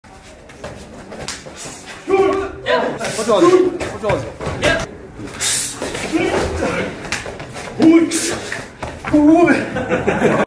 Wang Kai push hands at the 2019 Dongtai Practical Method Workshop
Daqingshan Instructor Wang Kai push hands at the 2019 Dongtai Practical Method Workshop. This is a friendly challenge by a local master during a break at the workshop. The local master is Li Yanjun, in yellow. Wang Kai is in red t-shirt.